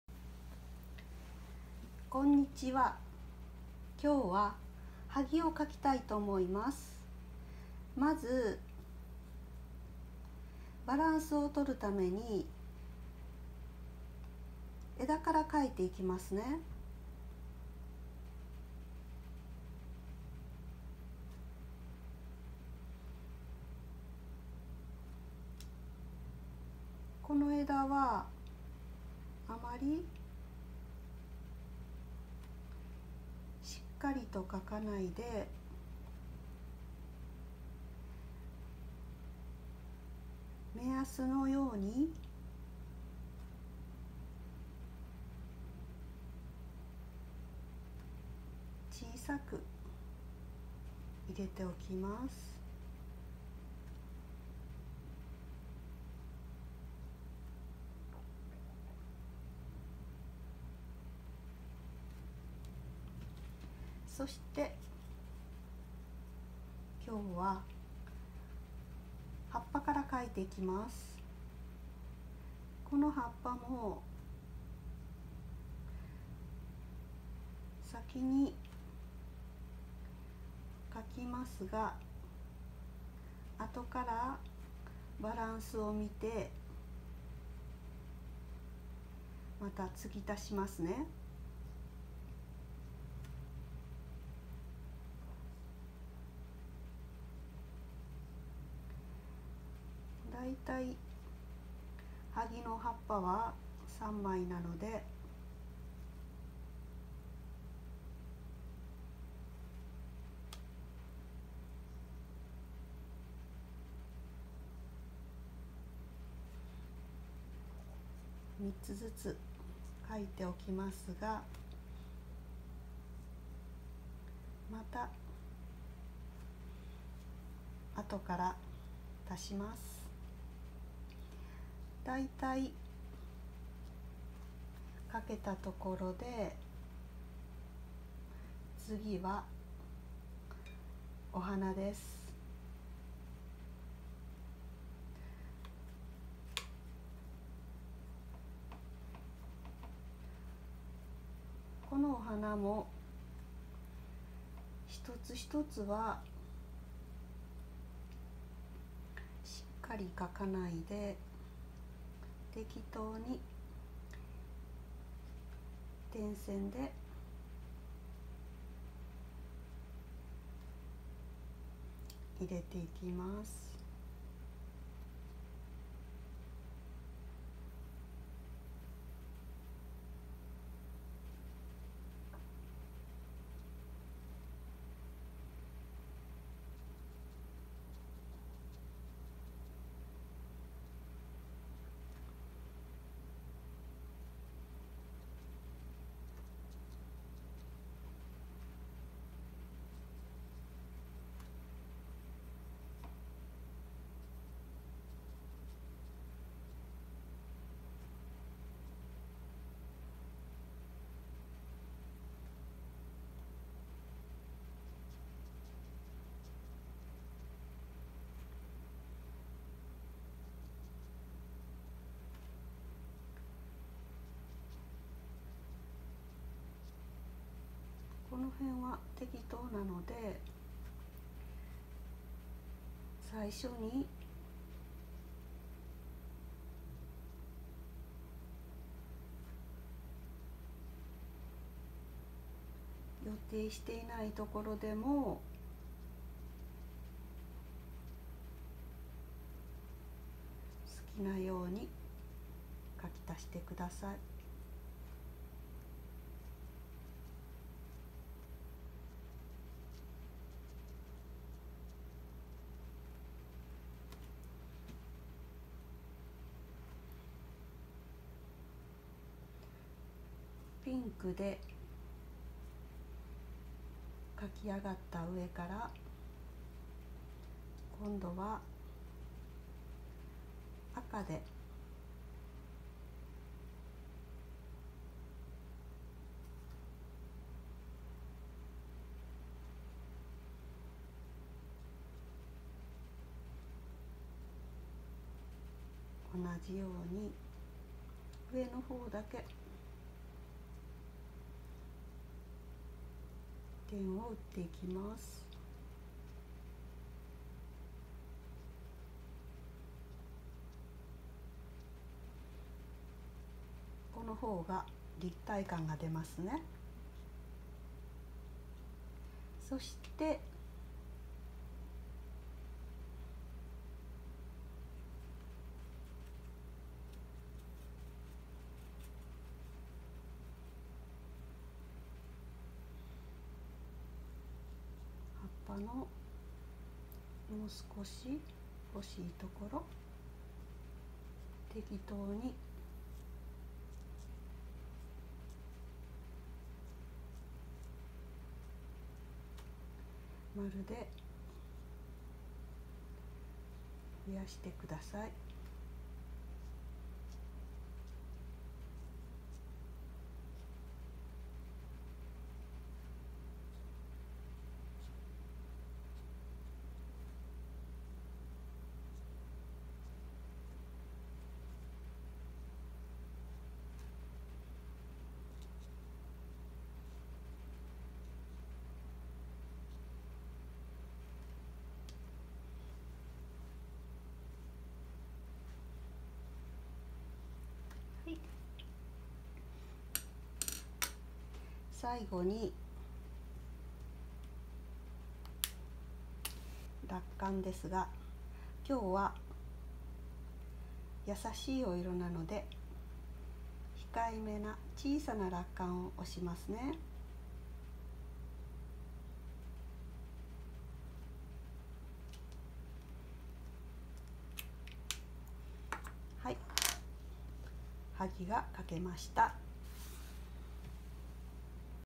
初めての人でも描けますハガキ絵💕【萩】花・水彩画Even the first person can draw easily [Hagi] watercolor
秋の七草【萩】を描きました💕
初心者の人お勧めの点画風。
バランスだけ考えればOKです🤗